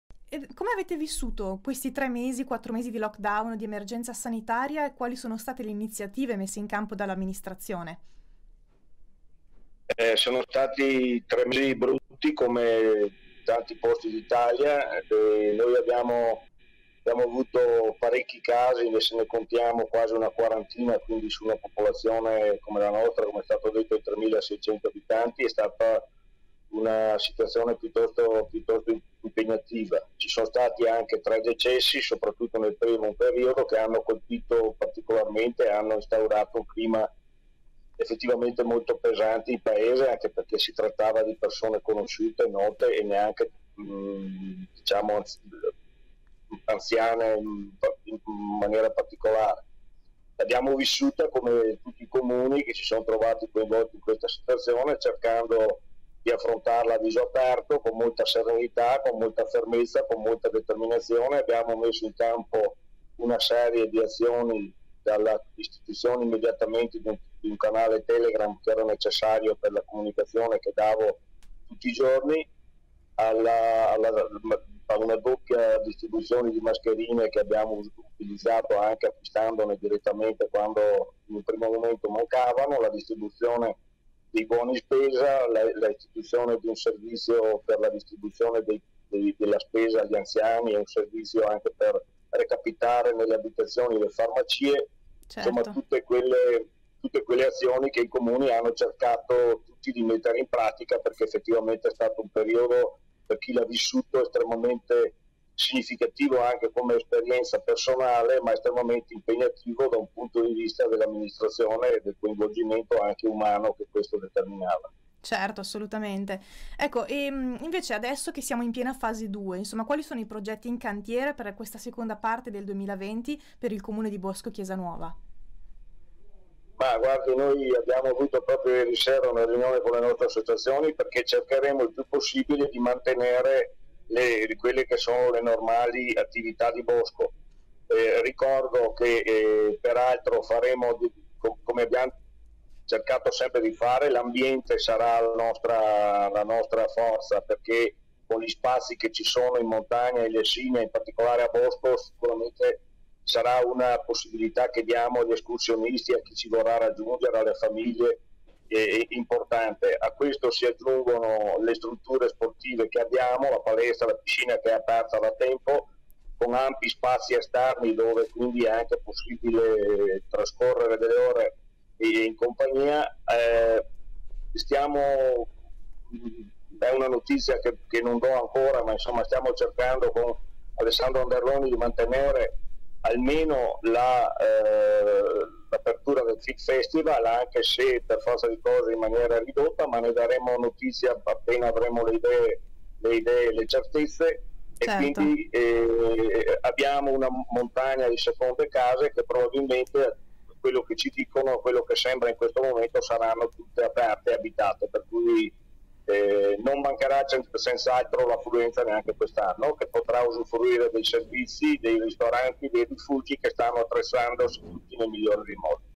Il sindaco di Bosco Chiesanuova: «La nostra forza sarà l'ambiente»
Claudio Melotti, primo cittadino di Bosco Chiesanuova, ha sottolineato l'importanza dell'ambiente montano per la ripresa nella fase post-covid.